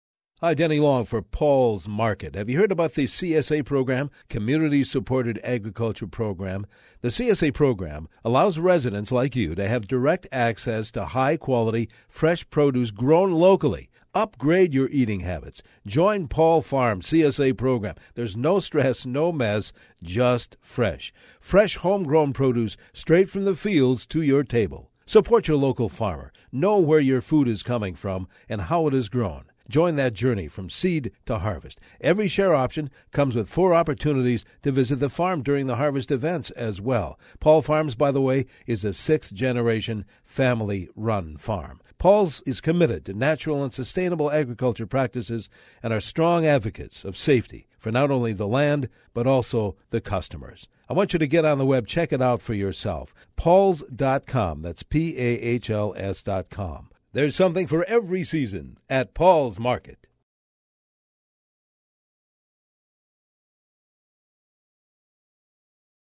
Minnesota CSA Program at Pahl's Market
Our Community Supported Agriculture program at Pahl's Market in Apple Valley, MN allows residents to have direct access to high quality, fresh produce grown locally. CSA members receive fresh homegrown produce straight from our fields to your table from mid-June through the end of October, as well as exciting opportunities to visit the farm on special Harvest Event days.
-As heard on Smart Gardens and endorsed by Denny Long of WCCO